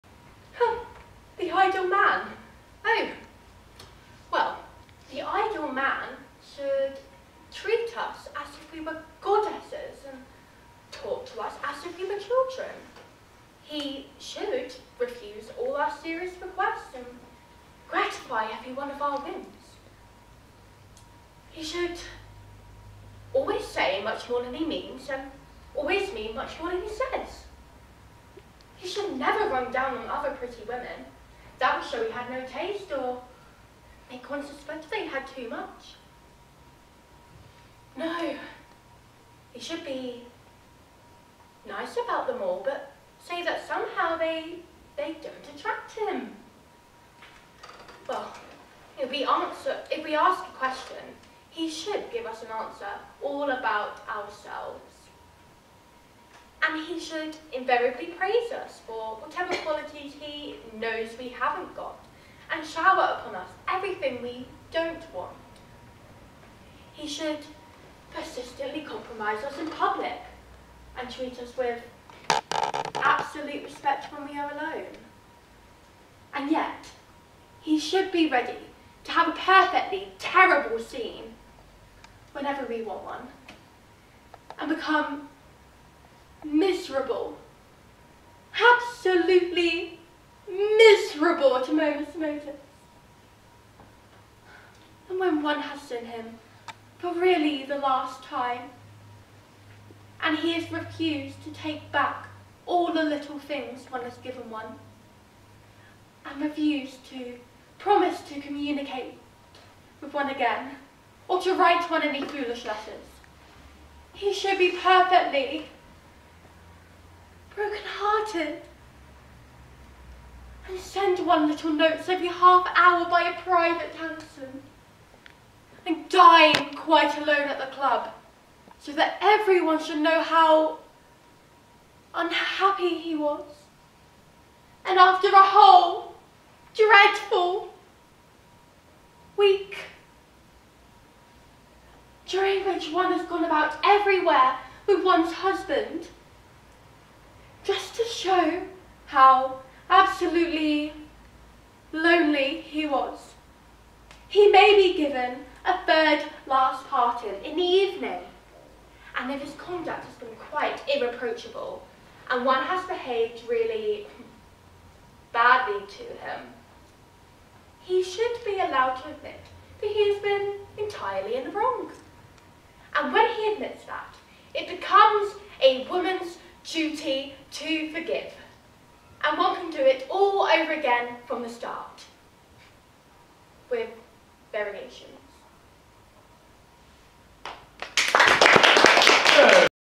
A Woman of No Importance | Mrs Allonby's Monologue | Rehearsal
A Woman of No Importance
Oscar Wilde
Mrs Allonby

My first rehearsal for my LAMDA Silver,
My final performance got Distinction (80/100 overall, 10/15 for interpretation, 6/10 for technique)